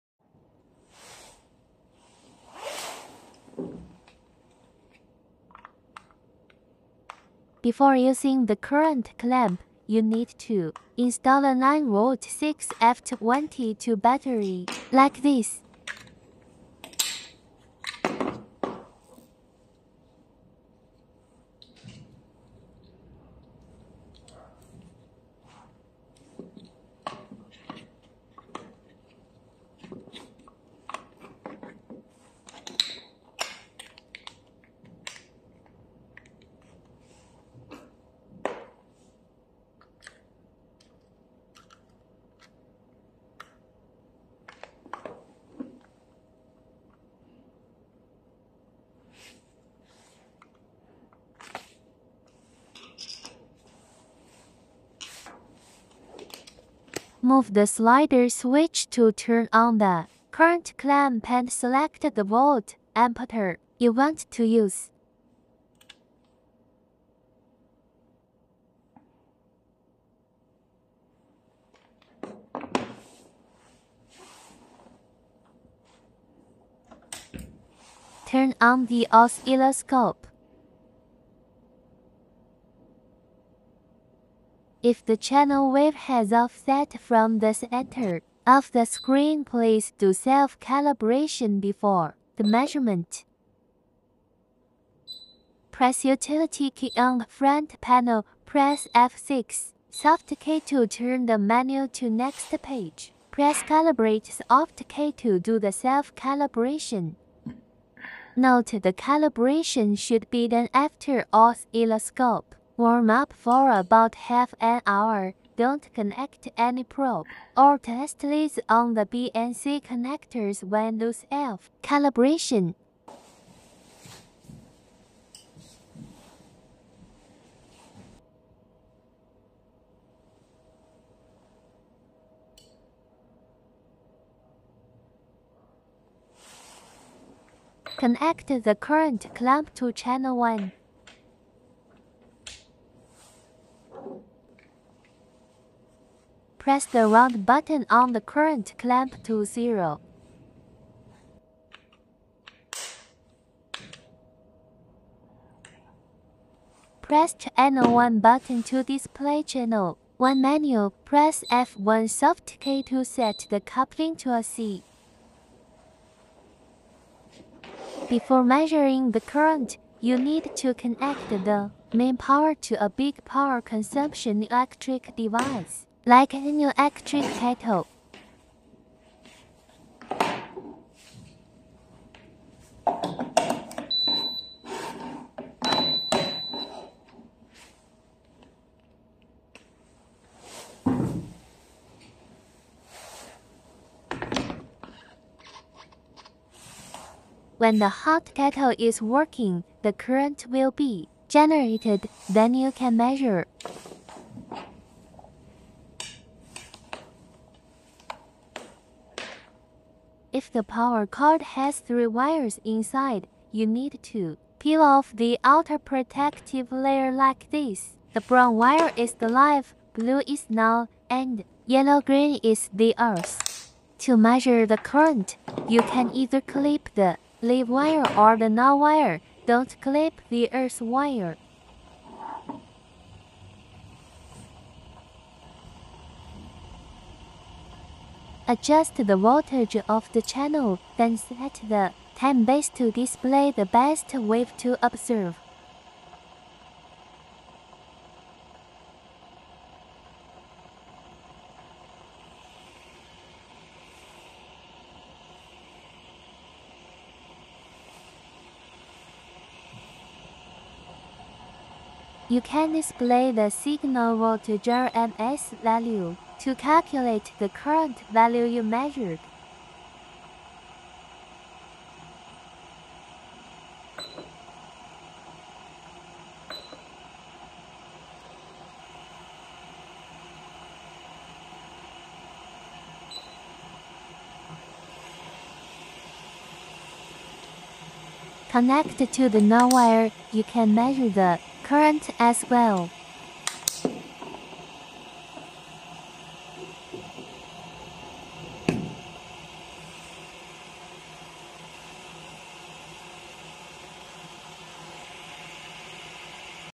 Use Oscilloscope and Current Clamp to Measure Current
Brief instruction about the usage of the current clamp.